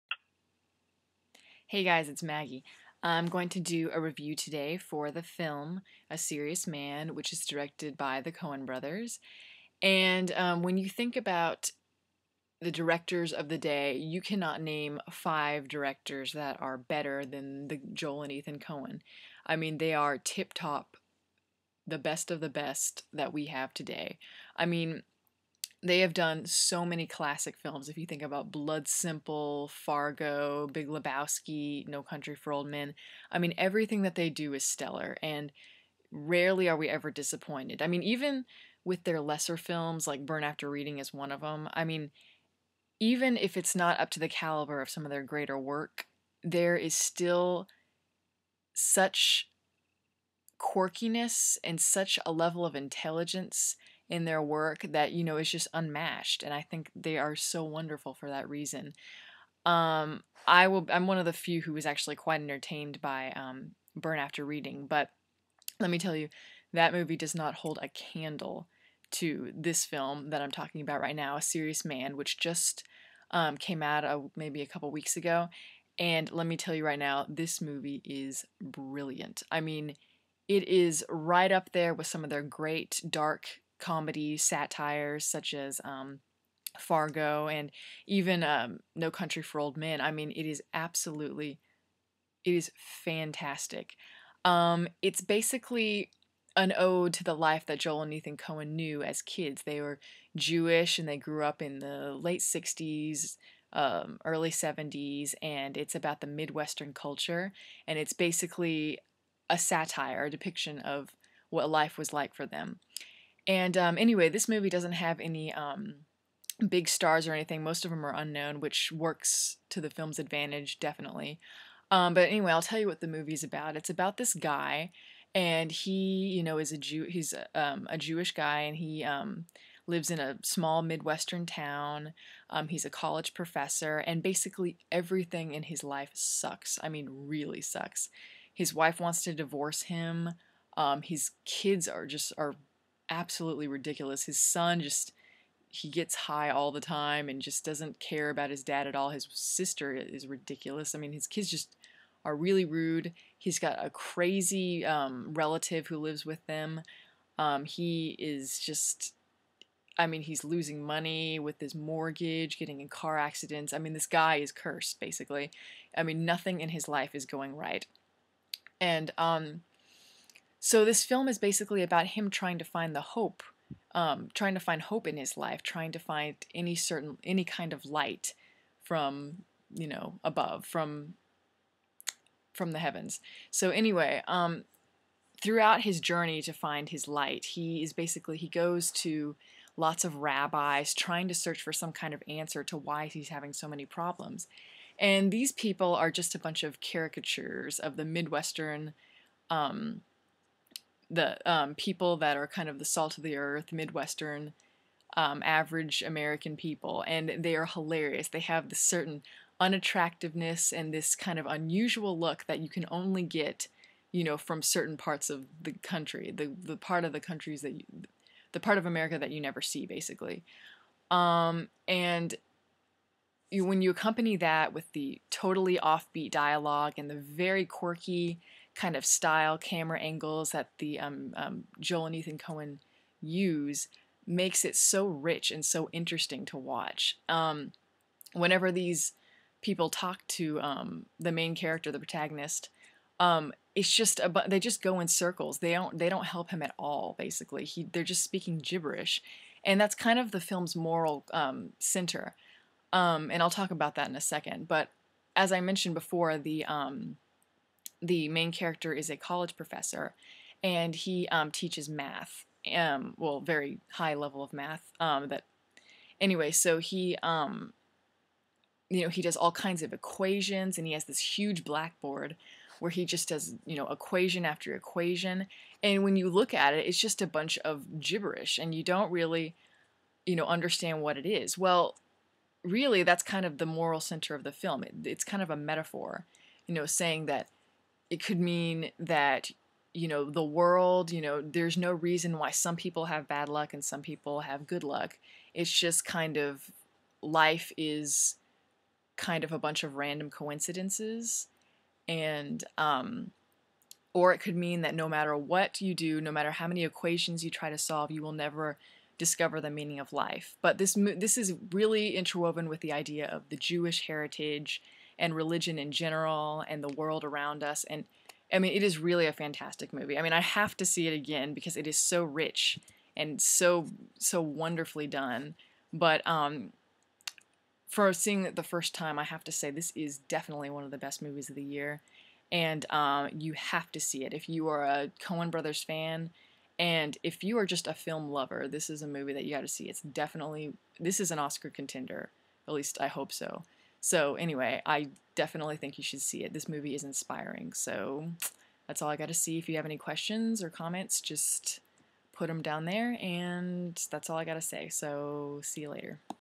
"A Serious Man" Movie Review (NON SPOILER)
My review of the film, "A Serious Man" written and directed by Joel and Ethan Coen.

starring Michael Stuhlbarg, Richard Kind, and Fred Melamed